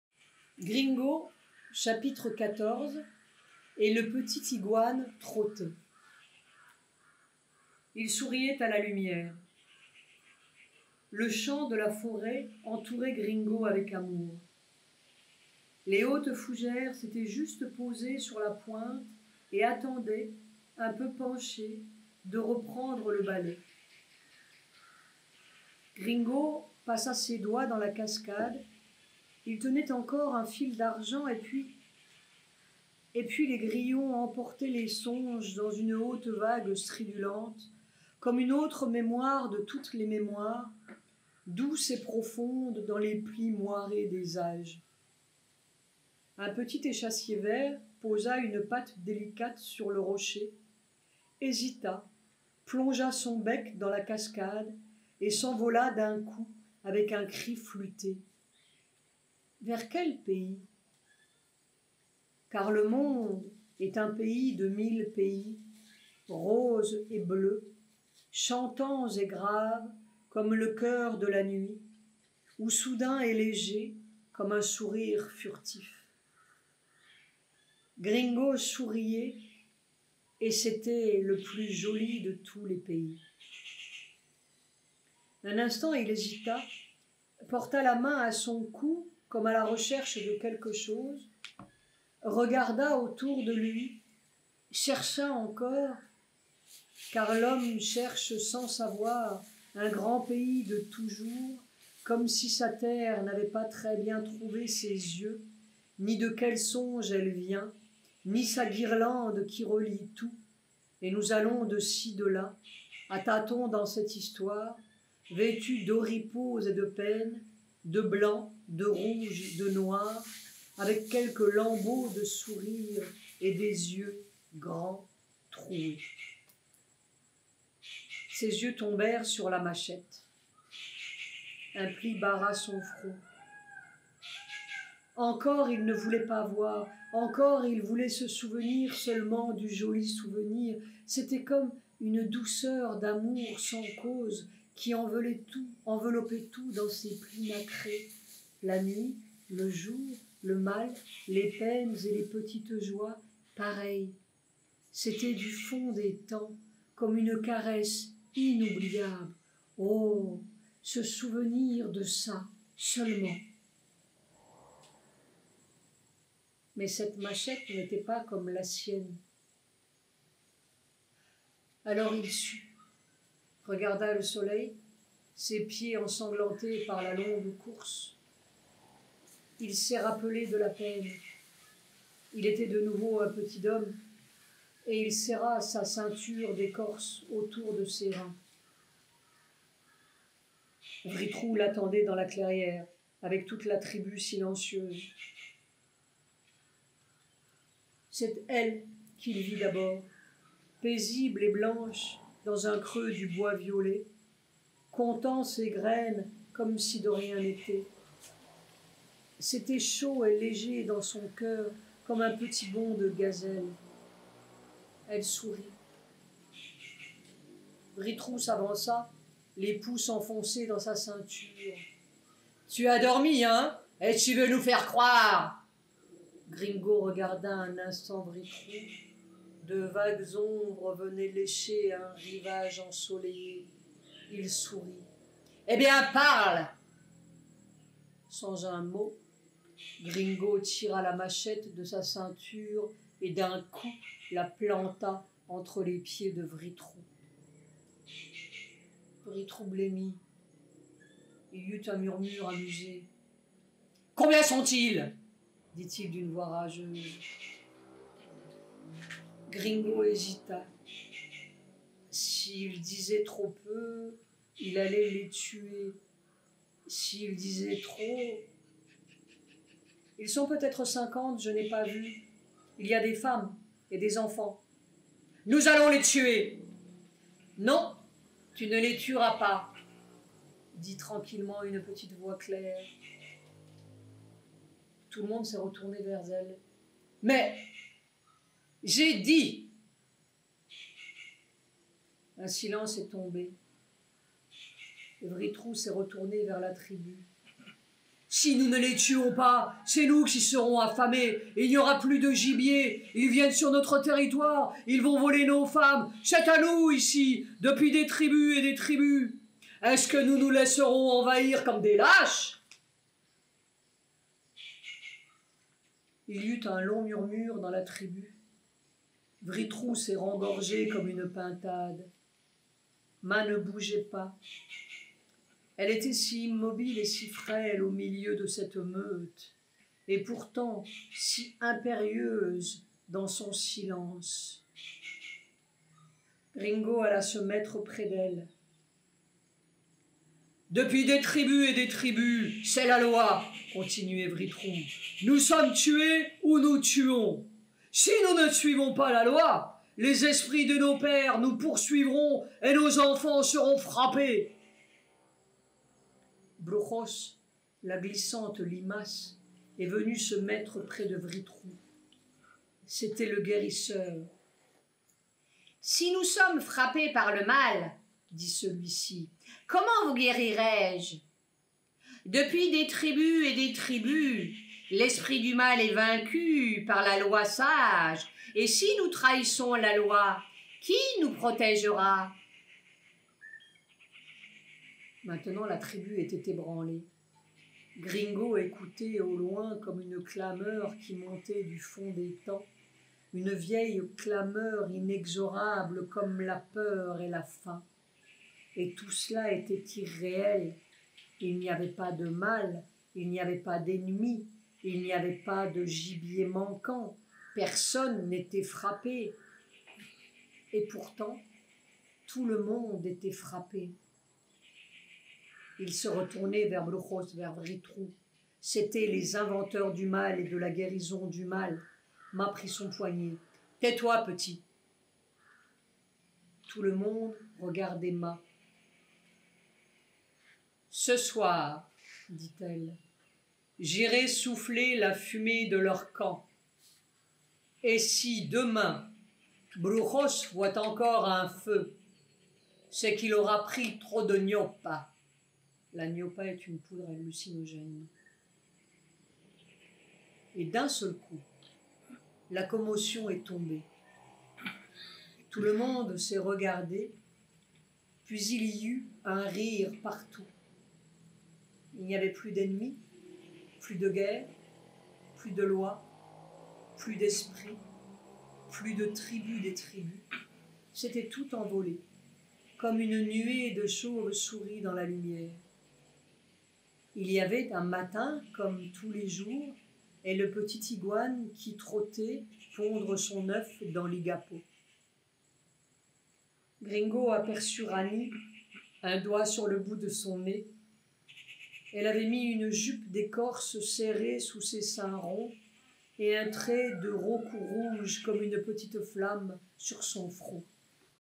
Satprem. GRINGO. Chapitre XIV. Et le petit iguane trottait.
Quelques précisions
1) Concernant l’identité des personnages
- Gringo est Satprem
- Mâ est Mère
- Rani est Sujata (compagne de Satprem de tant de vies)
- Quiño est Abhay Singh Nahar (frère de Sujata)
- Vrittru est Pranab
- Psilla est Pourna Prema (petite-fille de Mère)
- La Tribu est … l’Ashram
- Brujos … je n’ai pas deviné son identité.

2) Concernant la lecture
La voix change en fonction des protagonistes. Avertissement : la voix de Vrittru (qui apparaît pour la première fois au chapitre II) peut sembler très… agressive. Il suffit d’écouter l’enregistrement du 07 avril 1973 pour comprendre que la voix que vous entendez ici est en-deçà de l’original.

Nous sommes à « l’heure de Gringo ». Plus que jamais, c’est le moment de lire (ou d’écouter) cette merveille qui est, en fait, une synthèse de la Trilogie, comme mise en images, et où tout est dit. Toutes les clés sont là.
 L’écriture de Gringo est mantrique, comme l’est celle de la Trilogie. C’est-à-dire que chaque mot est non omissible car participant de la grande vague qui nous emmène, si seulement nous acceptons de nous laisser emmener, dans la grande forêt de Mère, dont Satprem dit :

"On ne peut pas transformer la forêt de Mère en un joli bois de Boulogne, ce n'est pas possible - autant demander au singe de parcourir le chemin mental en élaguant nos siècles de culture. Il faut parcourir le chemin. Ce n'est pas une recette ni une théorie, encore une fois : c'est une Action."
      (Carnets d'une Apocalypse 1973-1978, p.186)